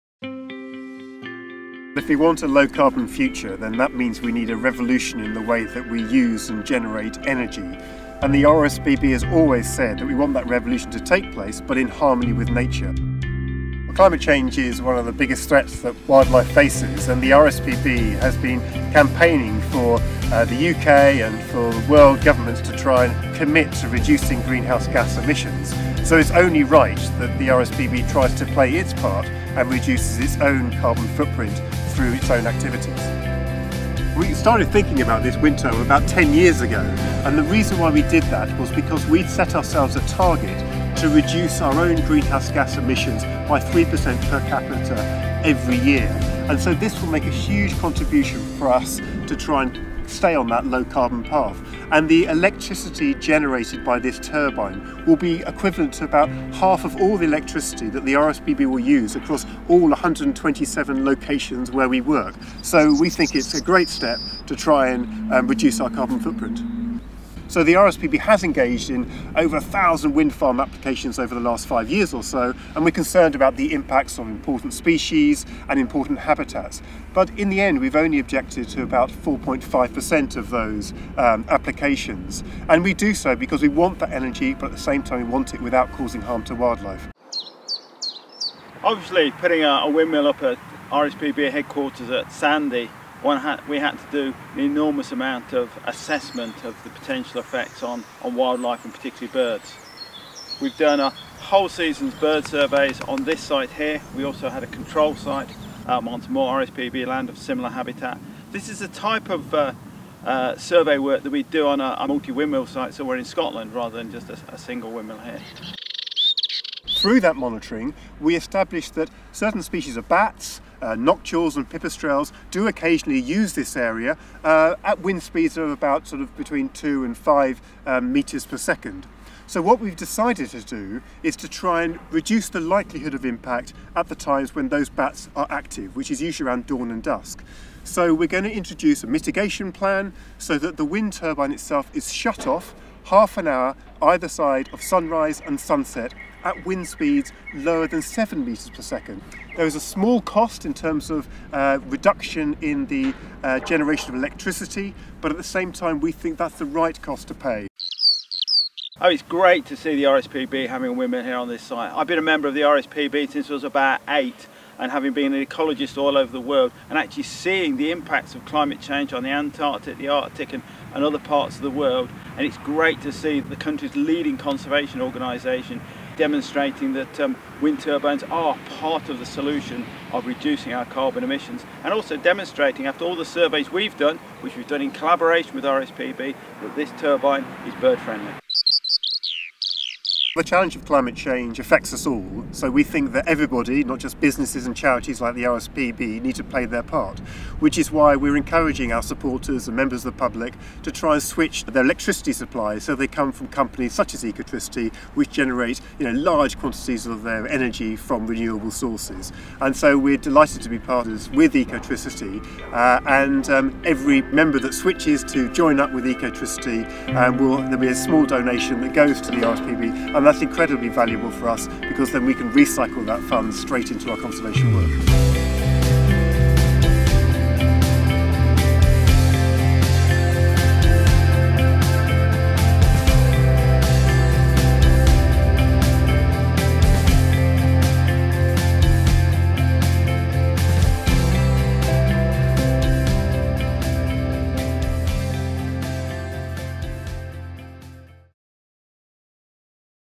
Constructing a wildlife friendly windmill for the RSPB – Documentary
We’ve completed construction of a new 100m windmill at the RSPB headquarters in Sandy, Bedfordshire. It will generate half of the RSPB’s total electricity needs across all of their 127 UK locations and will be bird and bat friendly – watch the video to find out how we did it.